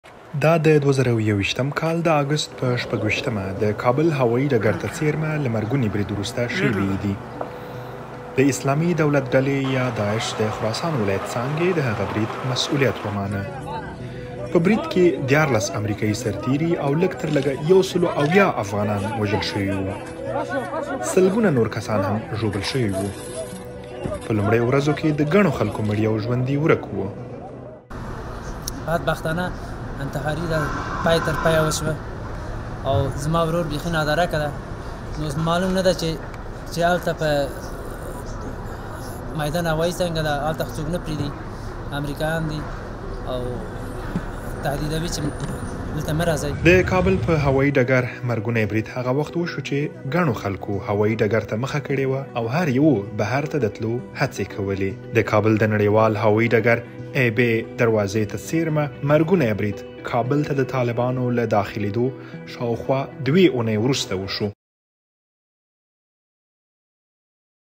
د کابل په هوايي ډګر د خونړي برید درېیم کال
د ۲۰۲۱م کال د اګست په ۲۶مه د کابل هوايي ډګر ابي دروازې ته څېرمه مرګونی برید وشو.
دا برید داعش کړی و چې په‌کې لږ تر لږه ۱۳ امریکایان او ۱۷۰ افغانان وژل شوي وو.